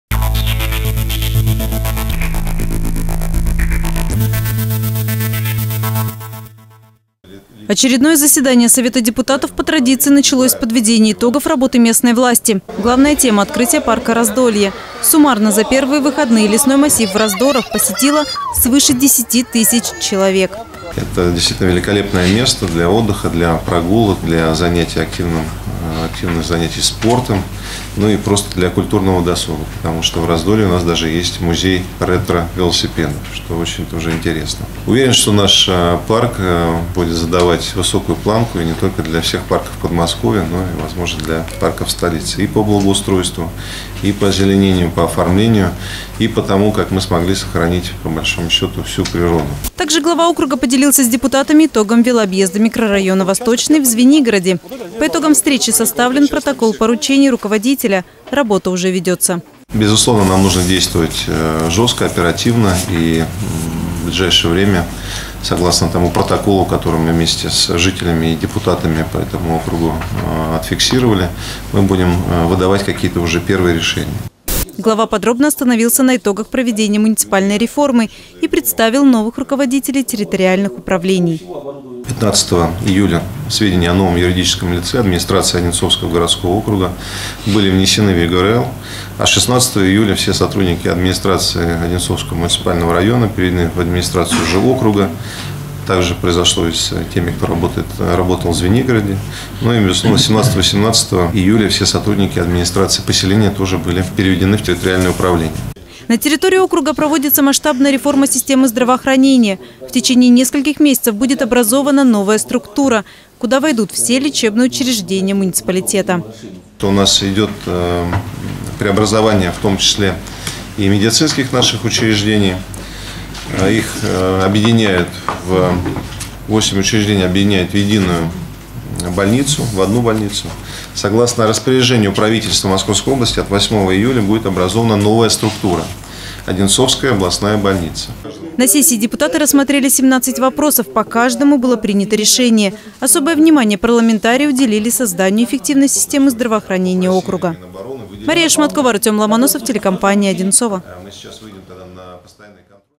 Совет депутатов Одинцовского городского округа
Основной вопрос повестки очередного заседания Совета депутатов Одинцовского городского округа – развитие здравоохранения муниципалитета, объединение всех лечебных учреждений территории в единую систему.